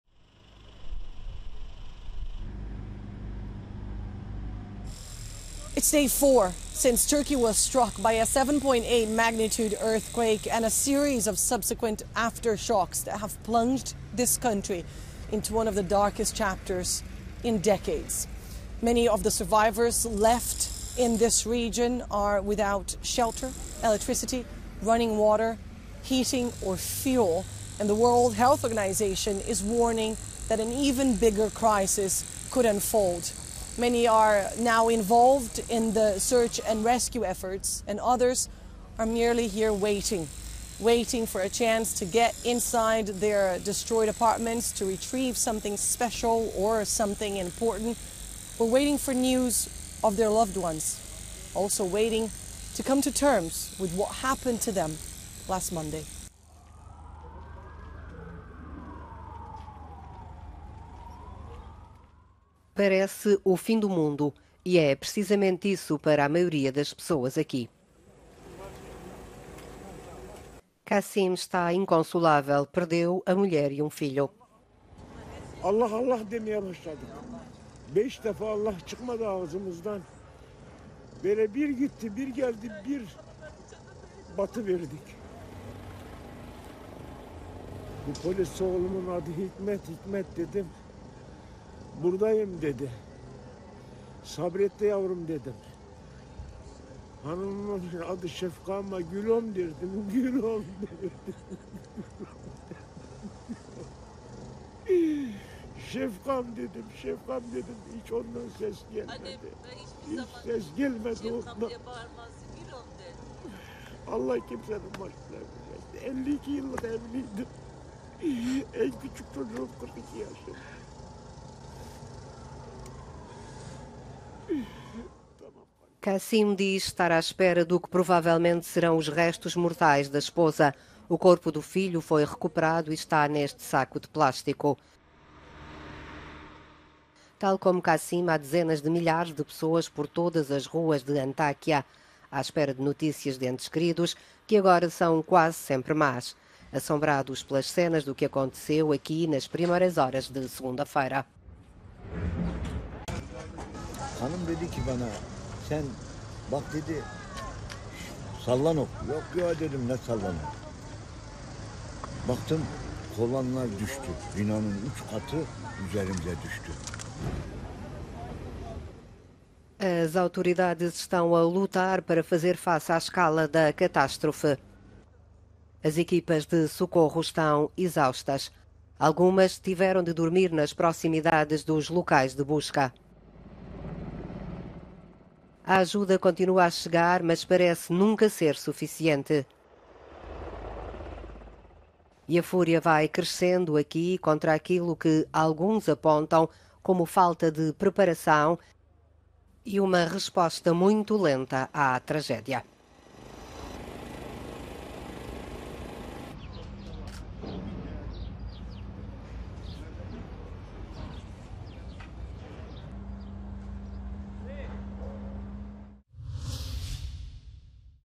Turquia: Sobreviventes desesperam por ajuda nas ruas da Antakya
A ajuda humanitária tem chegado lentamente à cidade de Antakya, na Turquia, onde milhares de pessoas desesperam desde a fatídica noite dos sismos